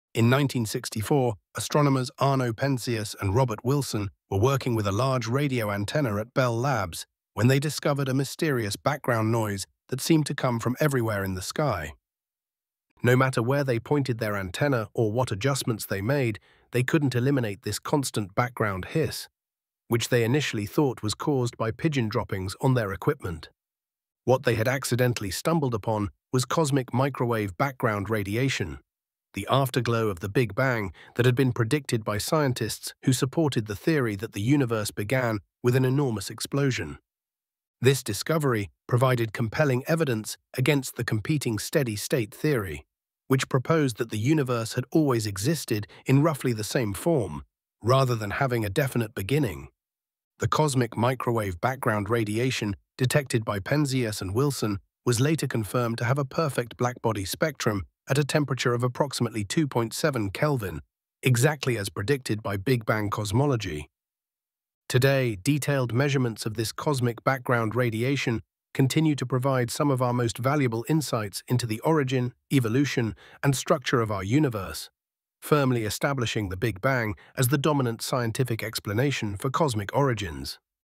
The Accidental Discovery that Changed Our Understanding of the Universe 🌌
In 1964, scientists Arno Penzias and Robert Wilson stumbled upon a mystery while at Bell Labs that would revolutionize our view of the cosmos. ✨🔭 This documentary dives deep into the remarkable discovery of cosmic microwave background radiation, uncovering how it serves as crucial evidence for the Big Bang theory. From vintage labs to modern observatories, join us as we explore the evolution of our universe!

🔍 Key moments covered include:
- The mysterious background noise detected in the 1960s
- The rivalry between the Steady State and Big Bang theories
- Advances in cosmic measurements that shape our understanding today.